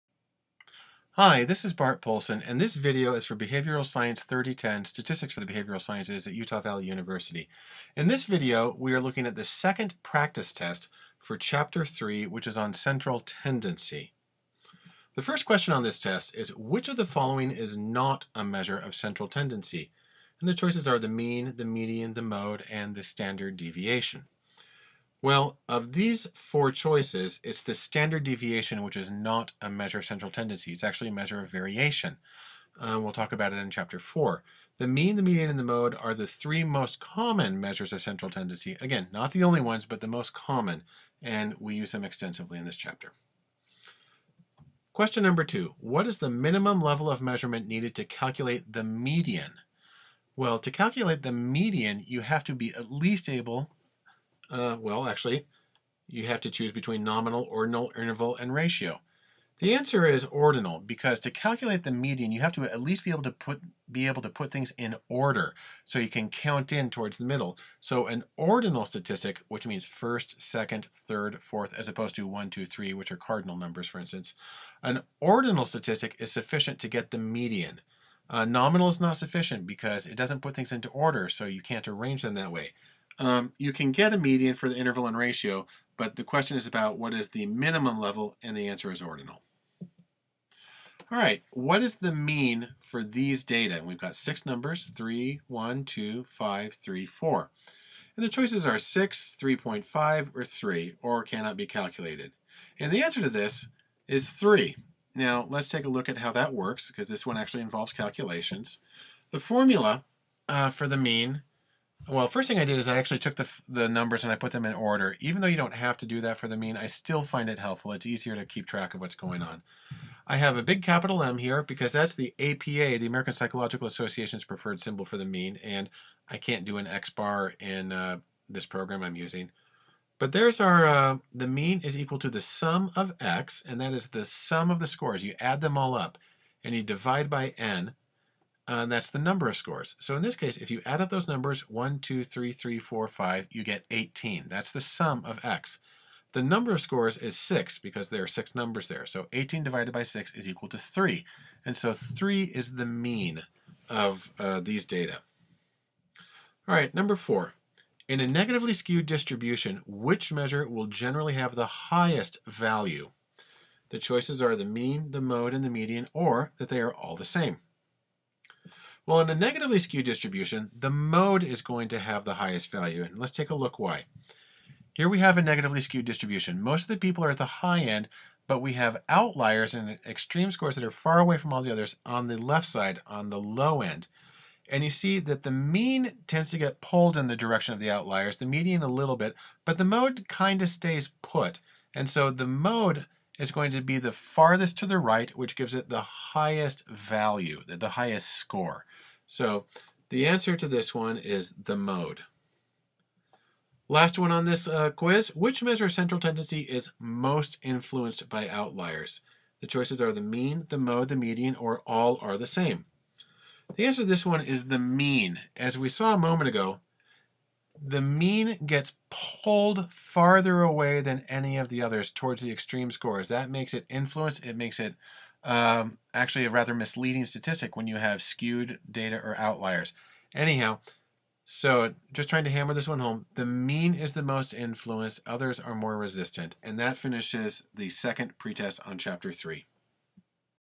Data Sense 03: Central Tendency / Review of Quiz 3
A review of one of four online quizzes Chapter 03: Central Tendency.

Please note that the quiz and questions in the video may be numbered differently but the questions should be the same.